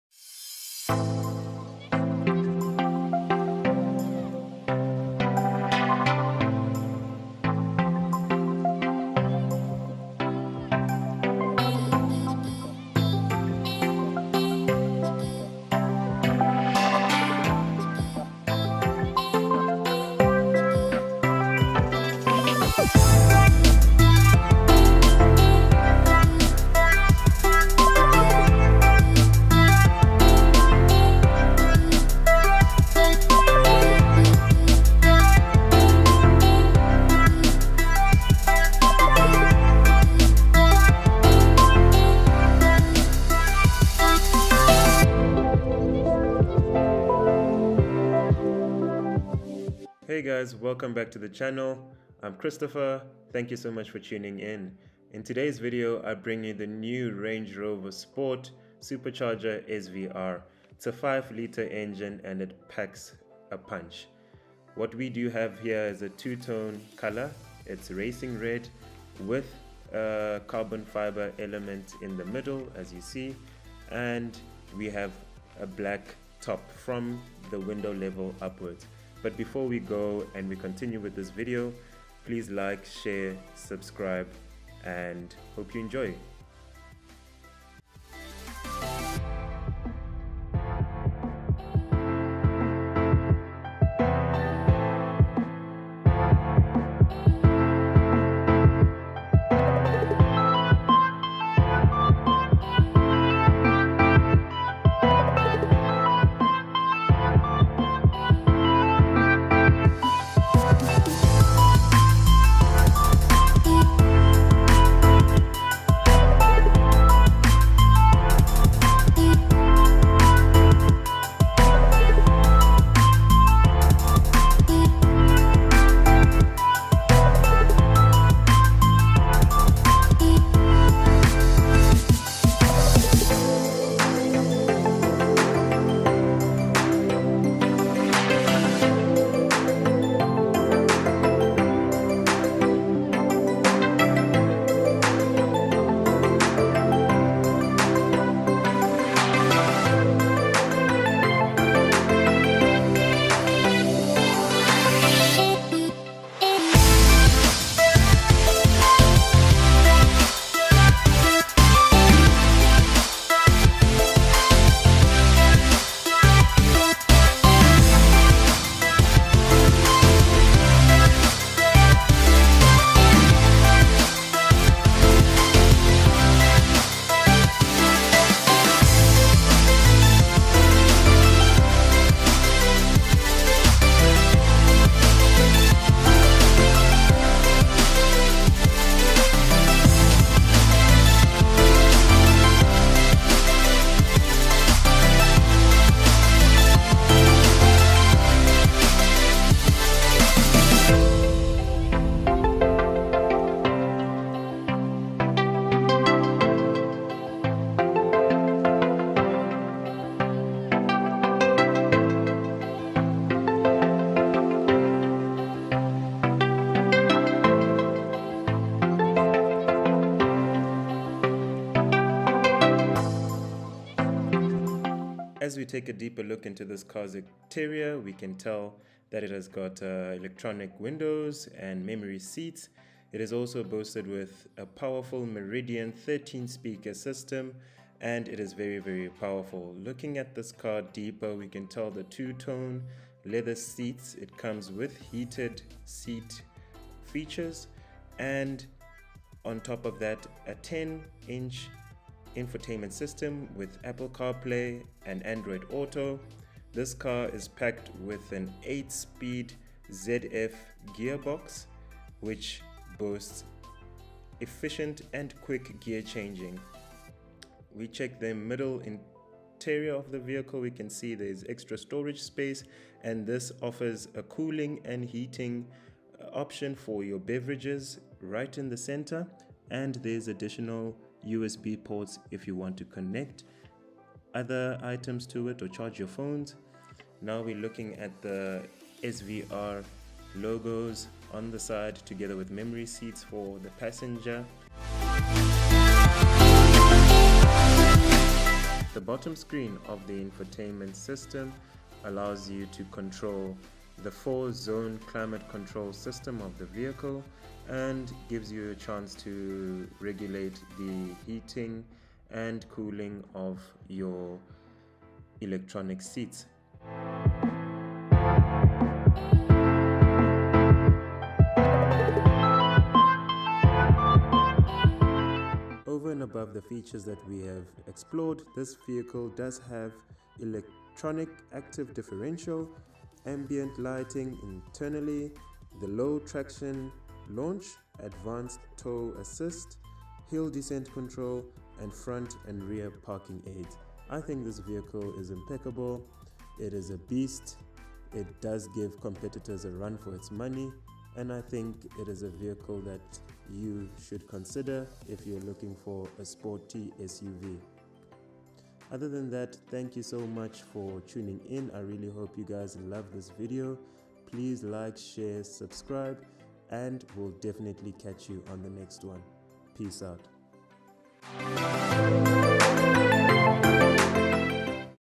2021 RANGE ROVER SPORT SVR 423KW | INTERIOR & EXTERIOR REVIEW
Range Rover Sport SVR delivers raw, visceral power from its 423kW V8 engine. Performance in all conditions is electrifying. Wr take a deeper look at what it has to offer.